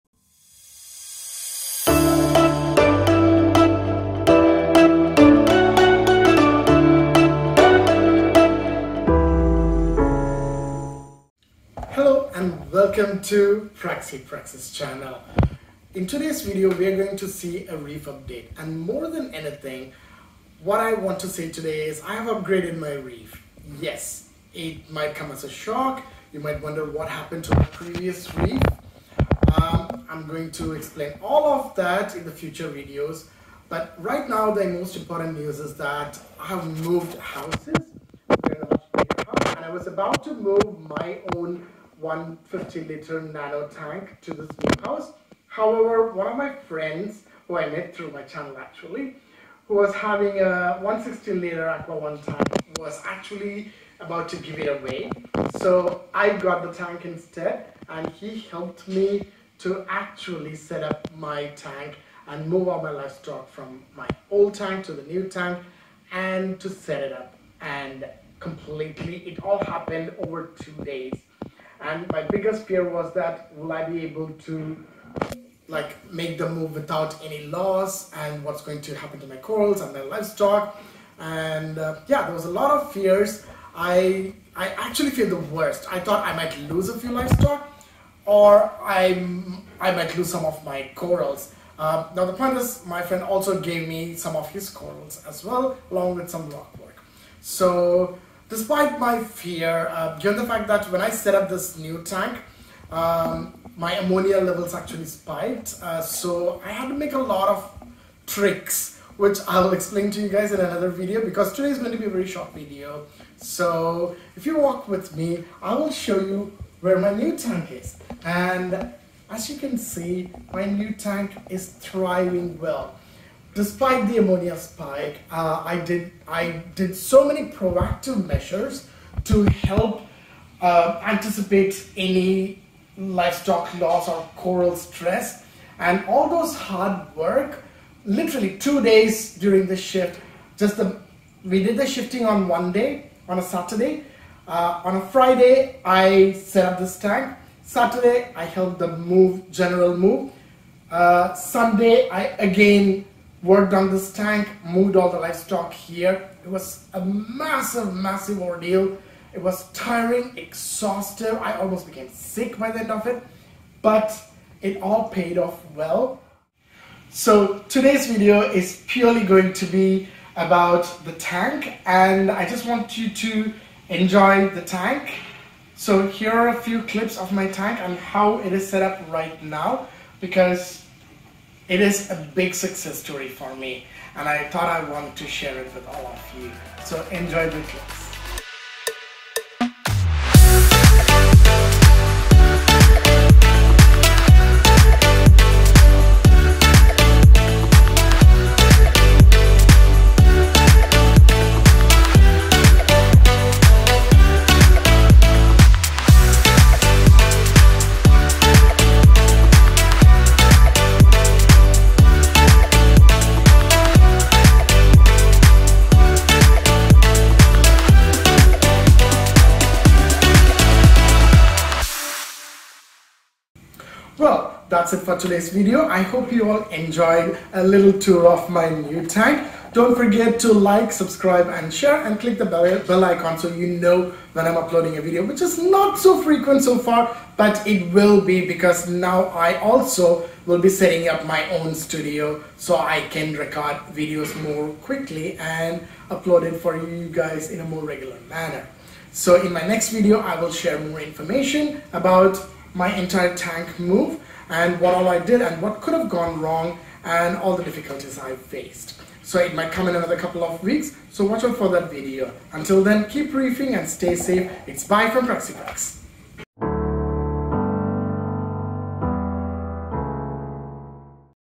New home - New Tank!!!
We moved and I managed to upgrade my tank to Aquaone Minireef 160 from a friend in the process. Here is a quick tour of my upgraded tank.